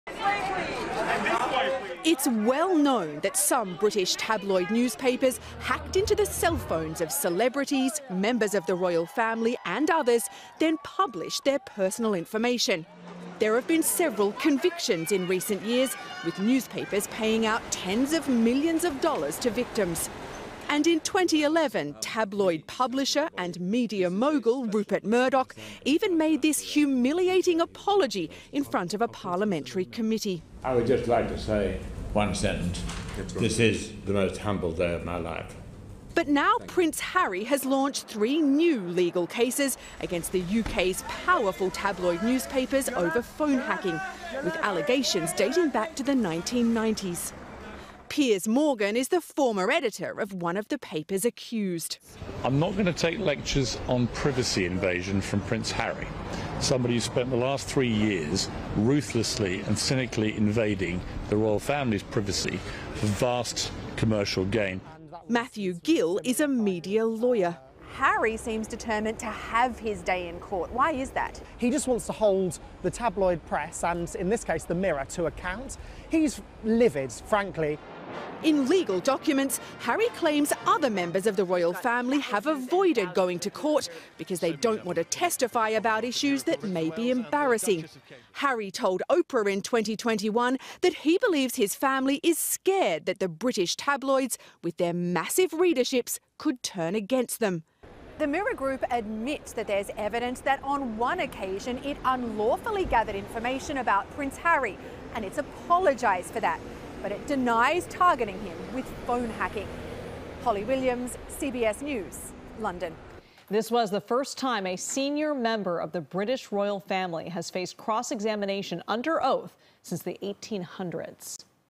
Prince Harry testifies in privacy invasion lawsuit against British tabloid publisher
Prince Harry took the witness stand Tuesday in his lawsuit trial against a British tabloid publisher accused of breaking the law for years to collect information about the royal family. Holly Williams reports on the controversy from London.